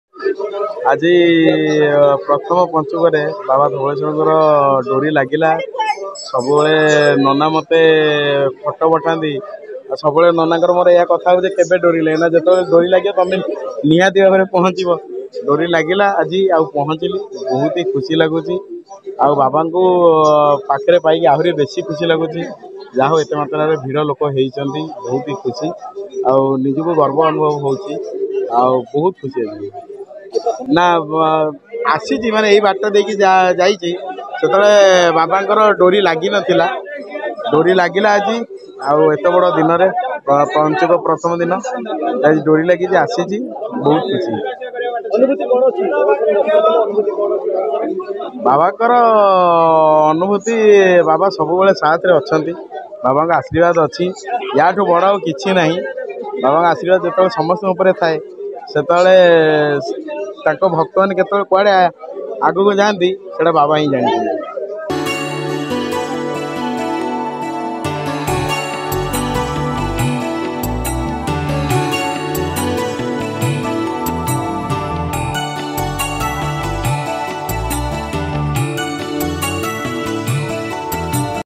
ବାବା ଧବଳେଶ୍ୱରଙ୍କୁ ଦର୍ଶନ କଲେ ହାସ୍ୟ ଅଭିନେତା ଗୁଡୁ || Sakala Khabar
ବାବା ଧବଳେଶ୍ୱରଙ୍କୁ ଦର୍ଶନ କଲେ ହାସ୍ୟ ଅଭିନେତା ଗୁଡୁ